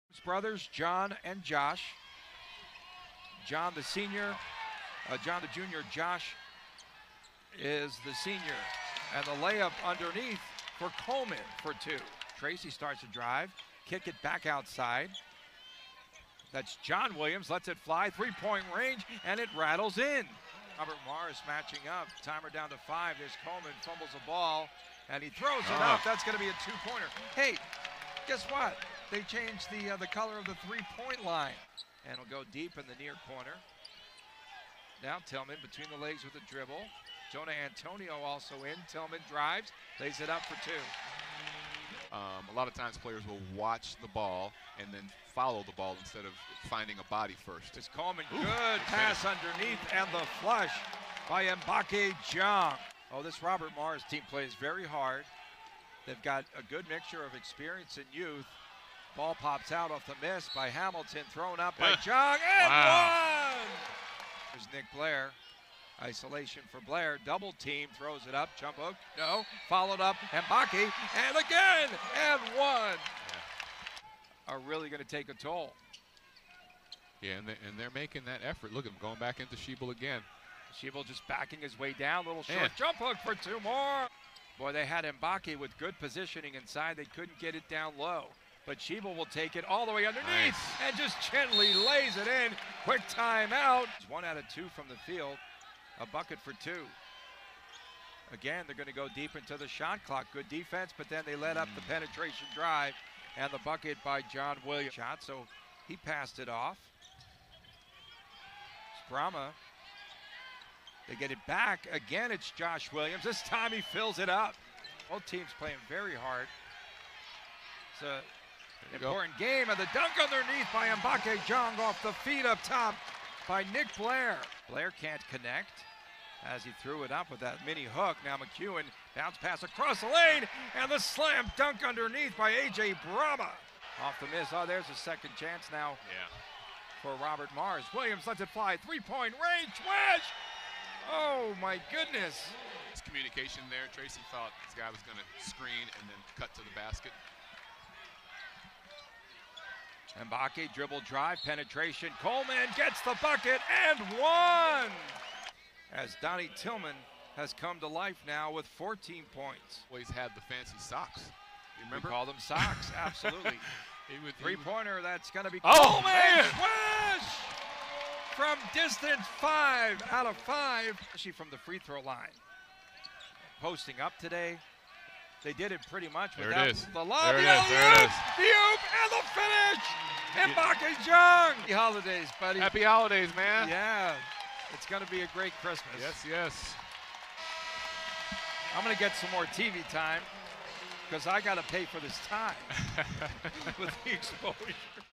UNLV vs. Robert Morris Basketball Highlights (2019-20) | Stadium
UNLV snapped a two-game skid with a victory in Las Vegas against Robert Morris. Catch all the highlights from a win for the Rebels right here!